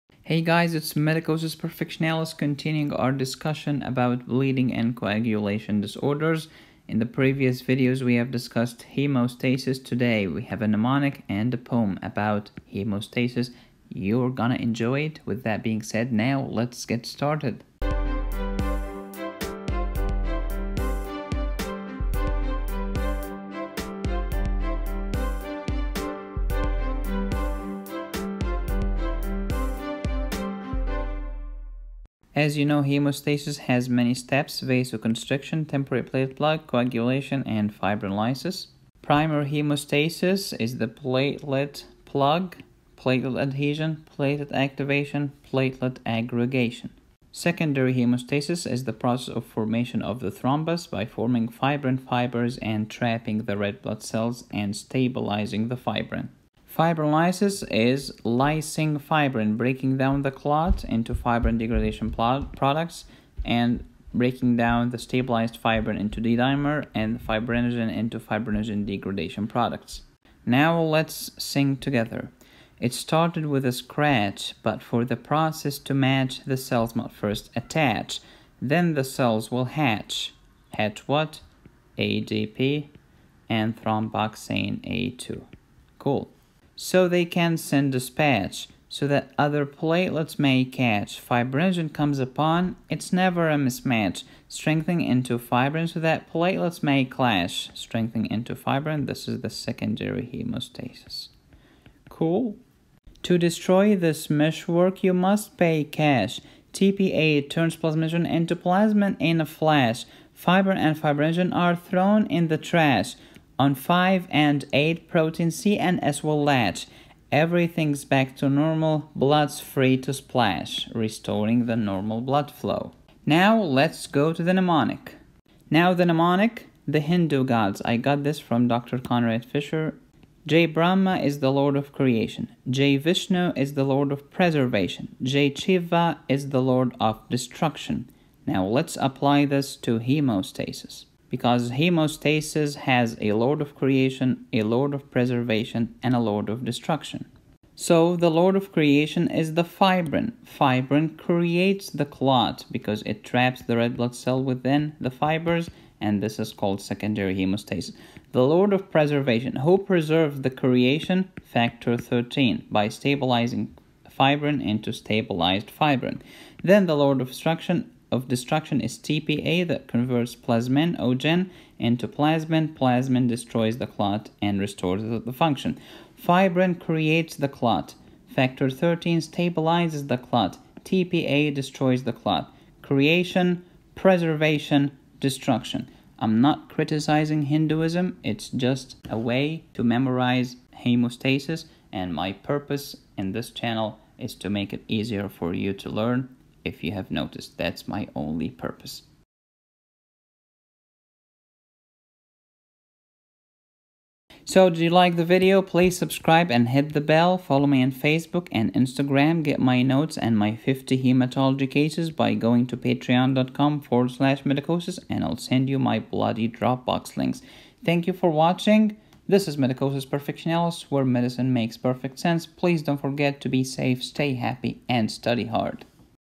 Hemostasis Mnemonic & Poem 😍 - Platelets and Coagulation - Hematology Series
Today, we have a mnemonic about hemostasis.
In this series, I have uploaded more than 30 videos about the topics of platelets, bleeding, and coagulation. If you like my videos, please consider leaving a tip